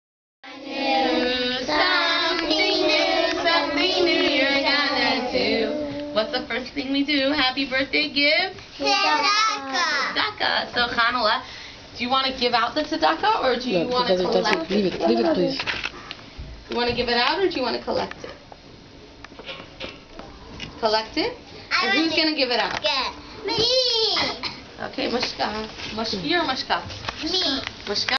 Chanale's Birthday Party יום הולדת של חנה'לה -Part I
Chanale's 4th birthday party in Beis Rivkah.
Rosh Chodesh Nissan